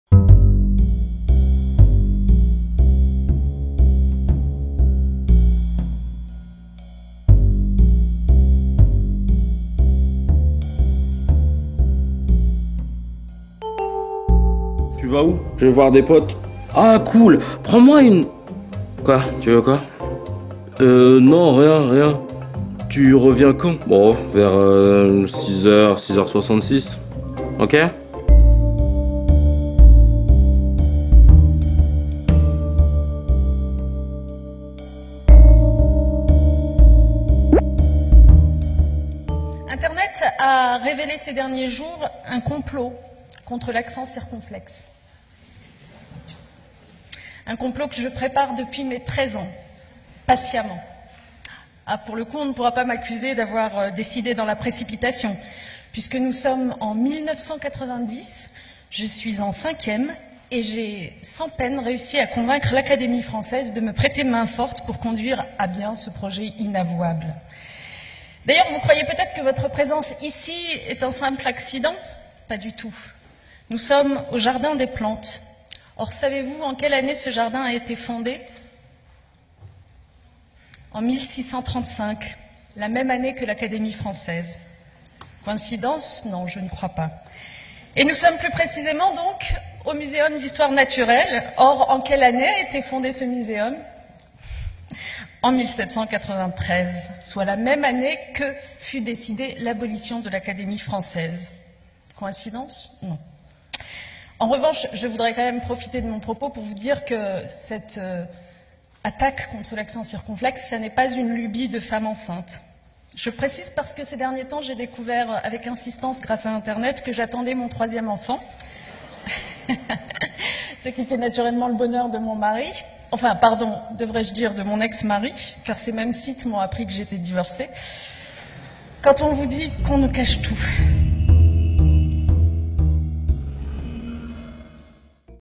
Le discours plein d'humour de Najat Vallaud-Belkacem face aux théories du complot
Dernier combat du gouvernement et notamment de la ministre de l'Éducation nationale, la lutte contre les théories conspirationnistes a fait l'objet d'une déclaration pleine d'ironie de la part de l'une de ses principales victimes.